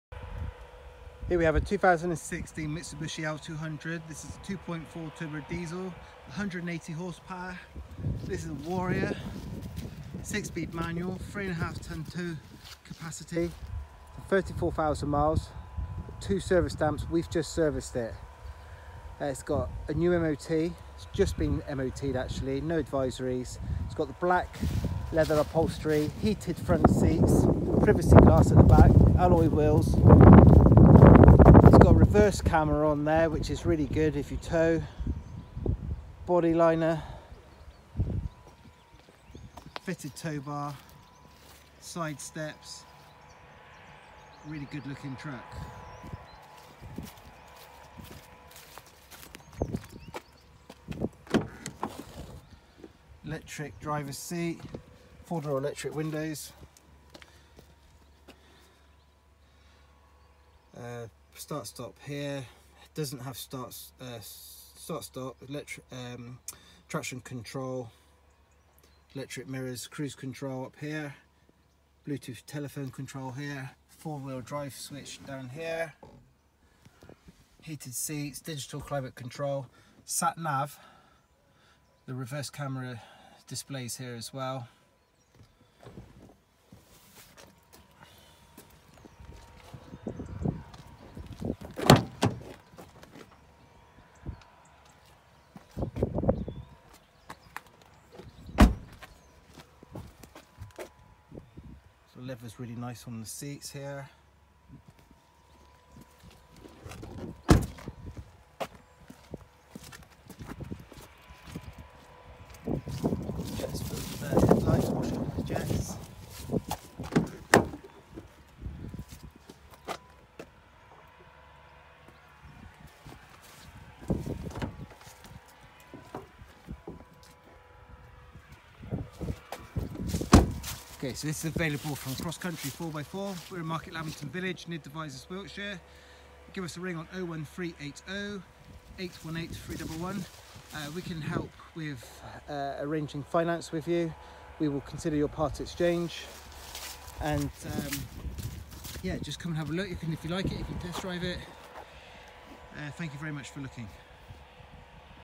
2016 Mitsubishi L200 2.4 DID Warrior
We are very pleased to offer this 2016 Mitsubishi L200 2.4 DI-D Warrior Double Cab 4WD, Finished in black with Heated Black Leather upholstery. Reverse Camera, Capable 3500 Kgs Tow Vehicle. Quotes for finance with options up to 5 years available. Please ring for further details., SD Card Navigation System, Cruise Control with Adjustable Speed Limiter, DAB Digital Radio, Dual-Zone Climate Control Air Conditioning, HD 7in Touch-Screen Display, Heated Front Seats, Reversing Camera, 17in Alloy Wheels with Locking Wheel Nuts, Automatic Rain Sensors Wipers, Bluetooth Hands-Free Telephone Connection, Drivers Powered Seat, Front and Rear Electric Windows, Hill Start Assist, Keyless Operation System with Engine Start/Stop Button, Multi-Info Display, Radio/CD with MP3 Compatibility, Bluetooth with Audio Streaming, Rear Windows Privacy Glass, Upholstery - Black Leather Seat Trim. 5 seats, Black, £